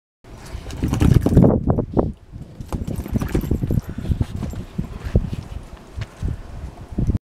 Dog daycare at white horse farm
Gunner playing with a ball at daycare